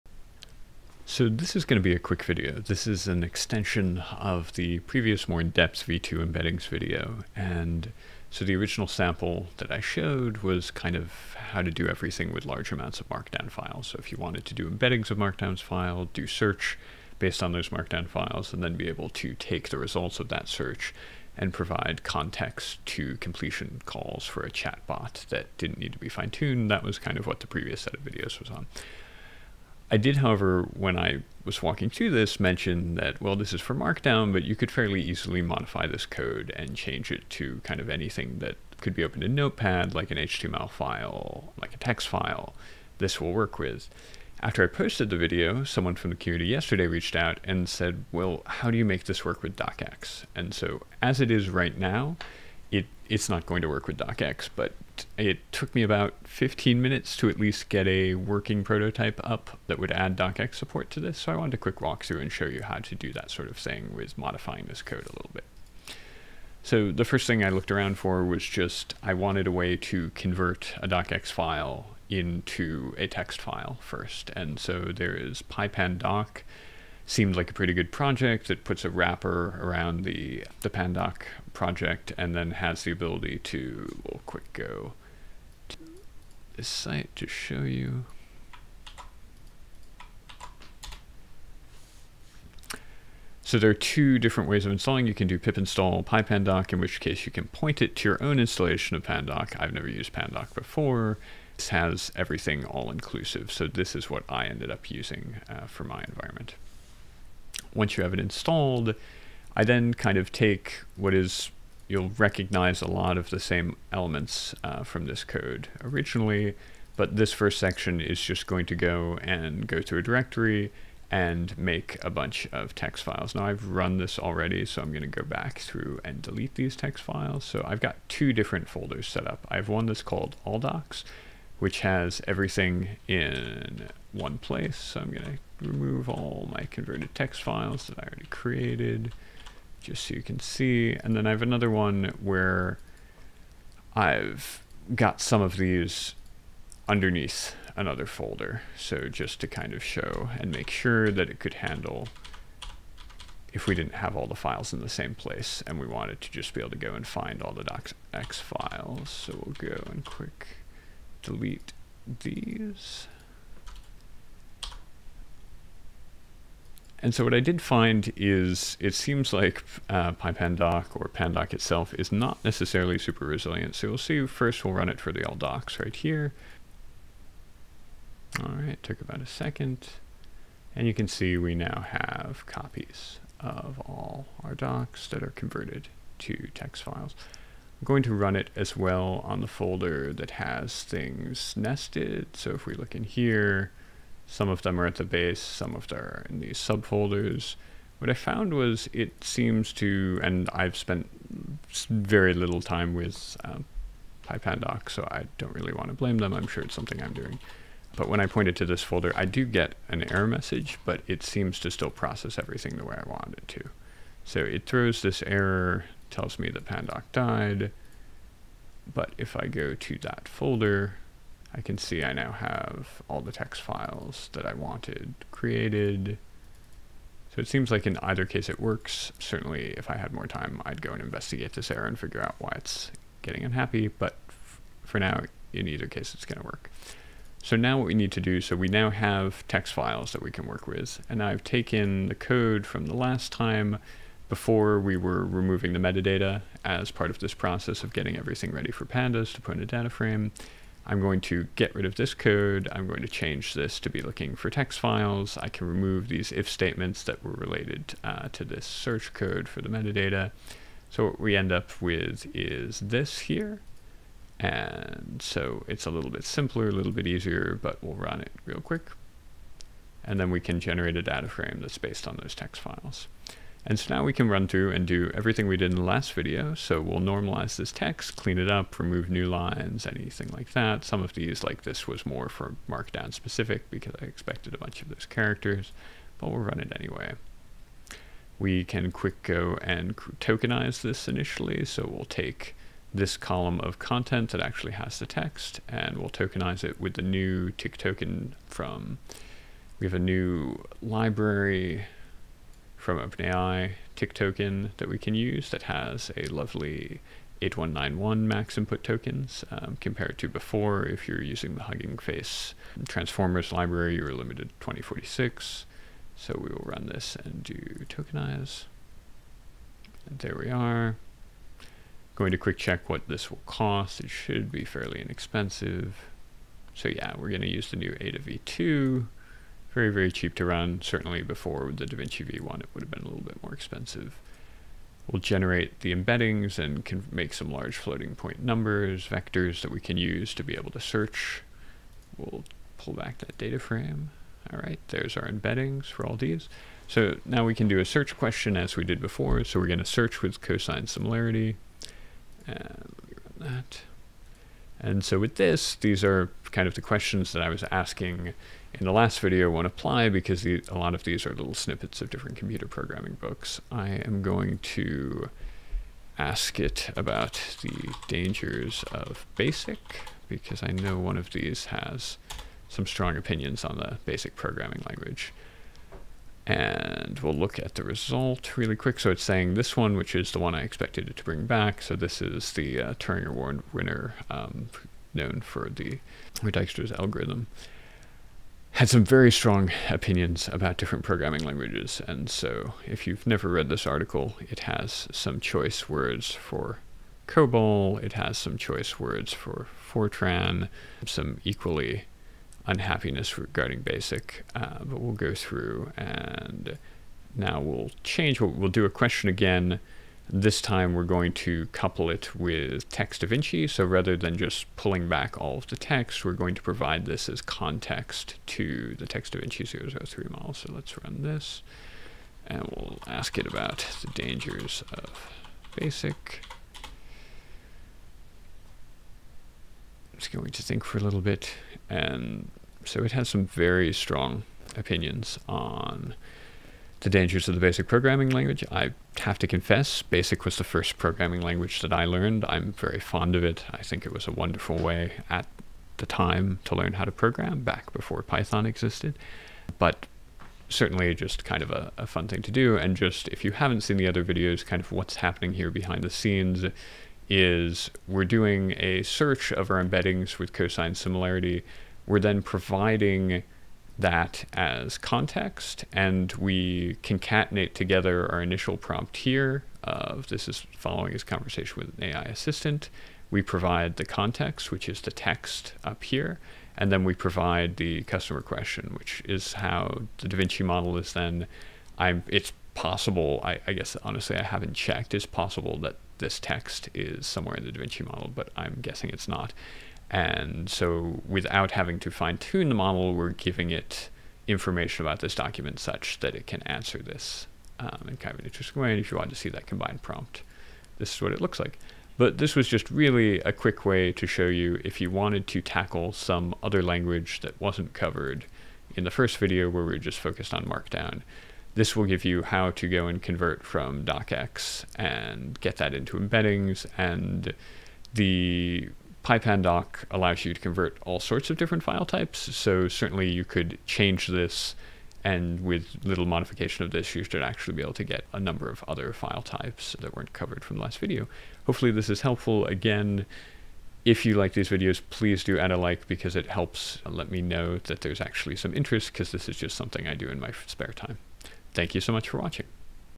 OpenAI v2 Embeddings & Search with .docx files
This is an follow on video to my earlier video on the new OpenAI v2 embeddings models

A community member had reached out and asked how to make my example work with Word Documents so this video is a quick walkthrough of how to bulk convert Microsoft Word documents from .docx to .txt and then follows the same steps of embeddings + search with cosine similarity and using the results to augment and provide context to a Davinci completion call.

While this video is specific to embeddings with Microsoft Word .docx files, the Python library used in my example will allow you to handle conversions of a large number of different document types to prepare them for OpenAI embeddings.

00:00 Intro
1:05 Convert docx to text with pypandoc
4:00 Basic pipeline for processing text files
4:30 Initial DataFrame is created
4:36 Basic data cleanup
4:48 Tokenization with tiktoken
5:30 Generate embeddings
5:46 Search with cosine similarity
7:05 Dangers of BASIC according to Dijkstra
8:21 Closing